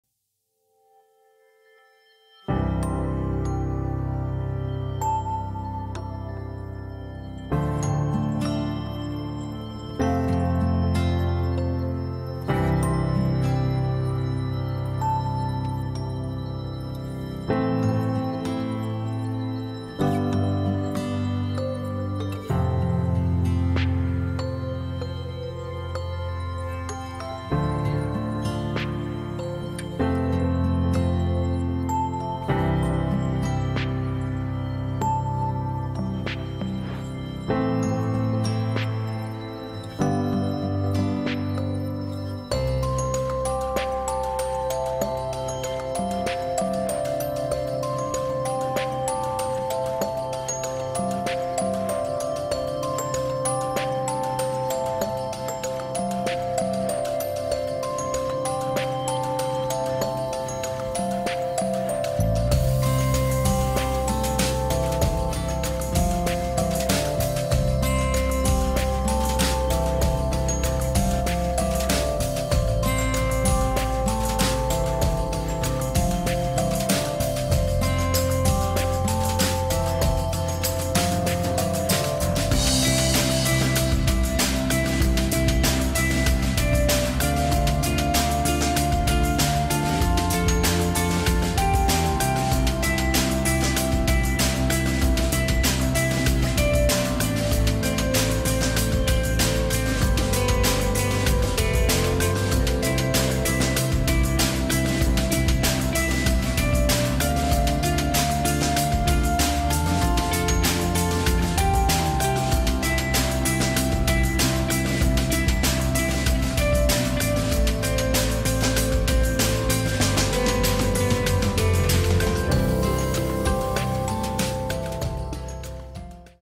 (MLP) SpeedPaint- Wierd faces! Fan art of Blits and Izzy!
I wanna make some spechal for Blits and izzy so i wanted to draw this!